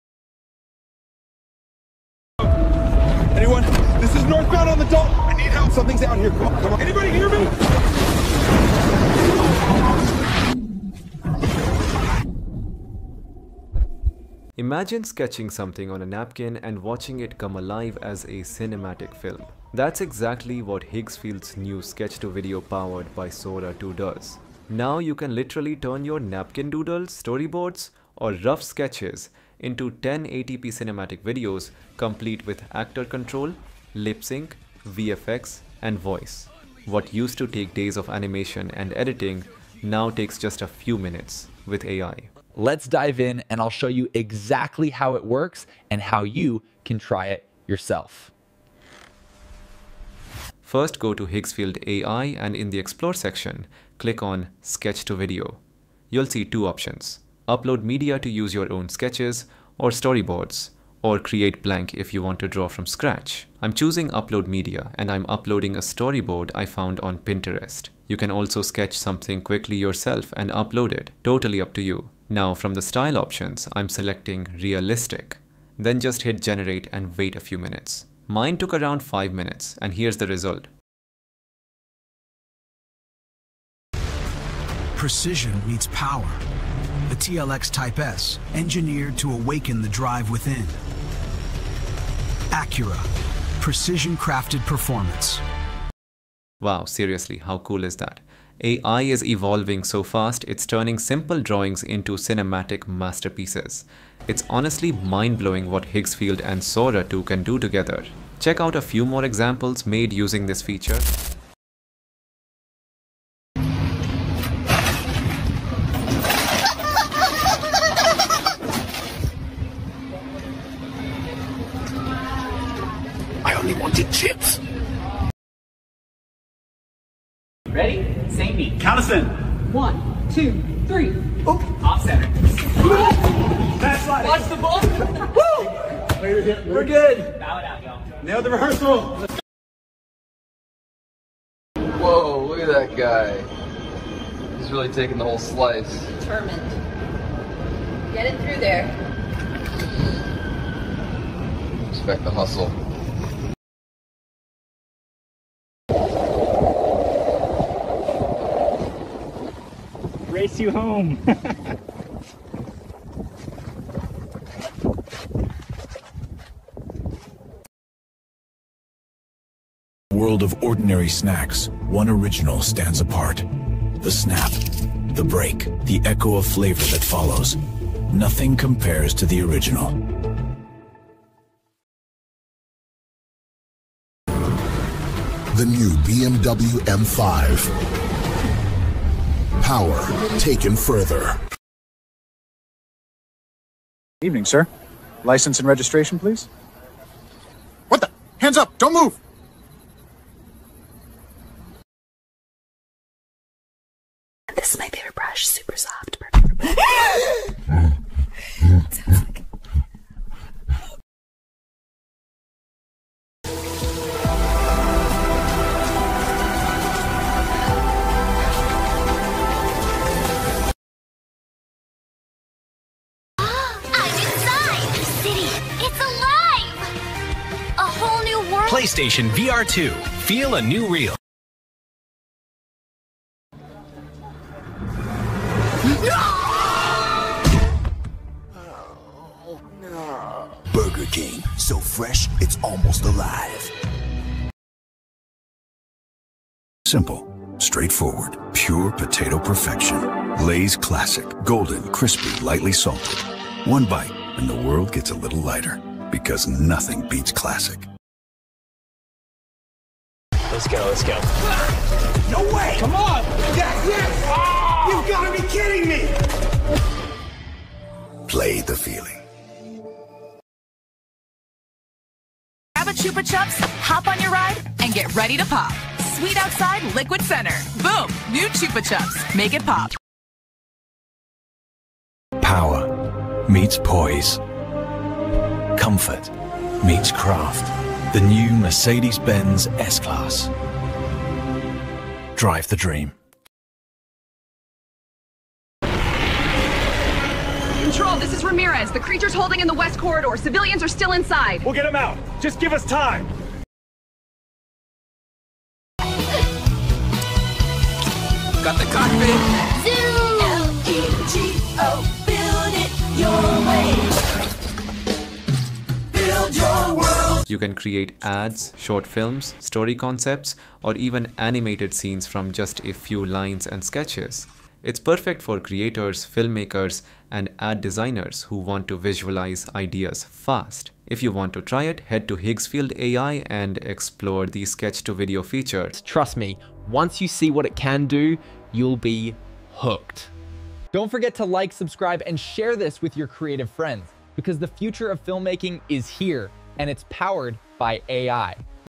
Higgsfield Sketch-to-Video: Turn Doodles into Cinematic Films with Sora 2 AI!
Imagine sketching something on a napkin and watching it come alive as a cinematic film — that’s exactly what Higgsfield’s new Sketch-to-Video powered by Sora 2 does. With this breakthrough AI tool, you can transform your doodles, storyboards, or rough sketches into 1080p cinematic videos featuring actor control, lipsync, VFX, and voice — all in just a few minutes. In this video, I’ll walk you through exactly how to use Higgsfield AI’s Sketch-to-Video feature step-by-step, from uploading your own drawings to choosing a visual style and generating stunning results. You’ll also see real examples created using this tool — perfect for creators, filmmakers, designers, and anyone exploring AI-powered video generation.
Try it out yourself on Higgsfield AI, and experience how Sora 2 is redefining creativity and storytelling through artificial intelligence.